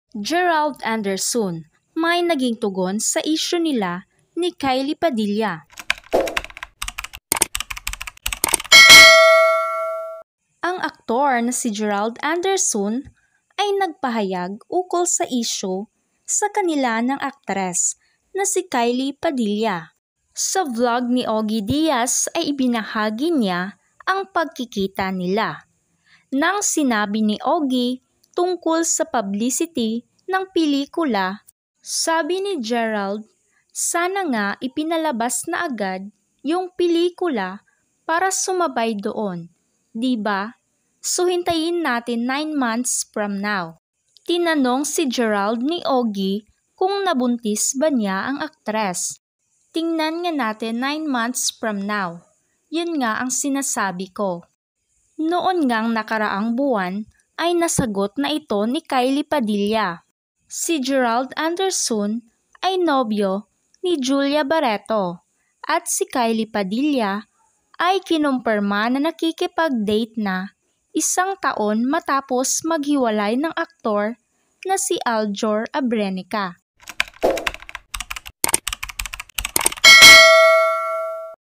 GERALD ANDERSON MAY NAGING TUGON SA ISSUE NILA NI KYLIE PADILLA
Ang video na ito ay tungkol sa sinabi ni Gerald.